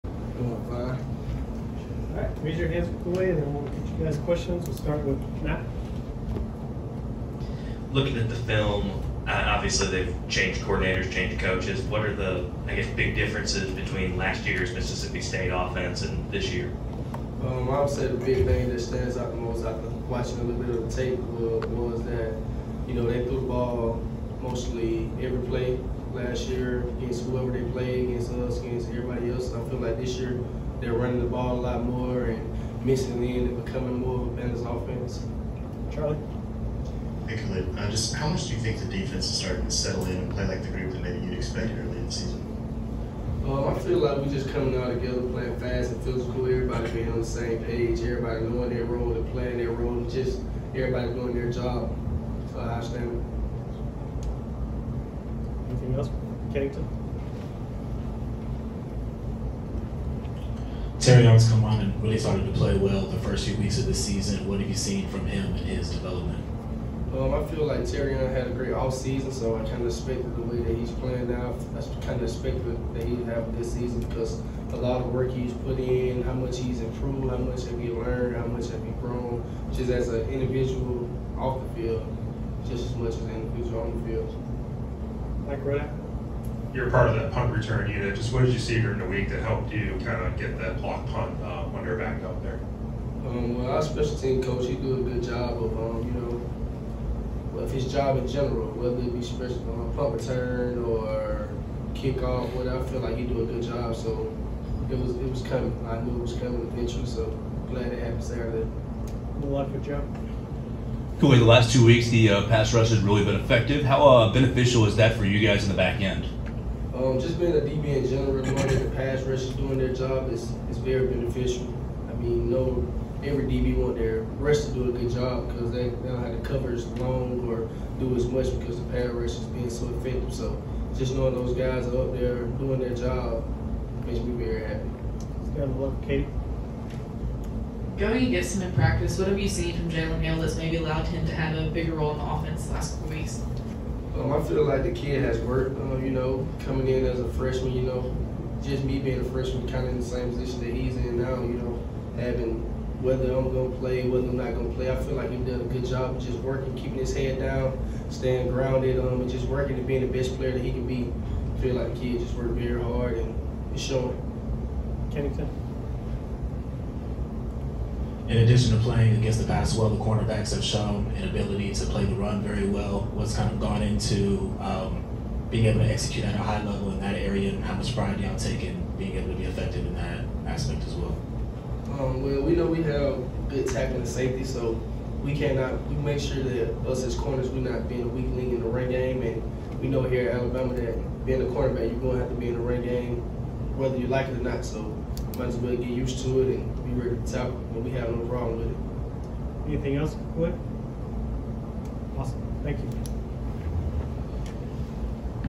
Kool-Aid McKinstry - Mississippi State Tuesday - Sept. 26, 2023
Alabama Crimson Tide cornerback Kool-Aid McKinstry meets with the media to discuss the Ole Miss victory and preview this weekend's matchup with Mississippi State.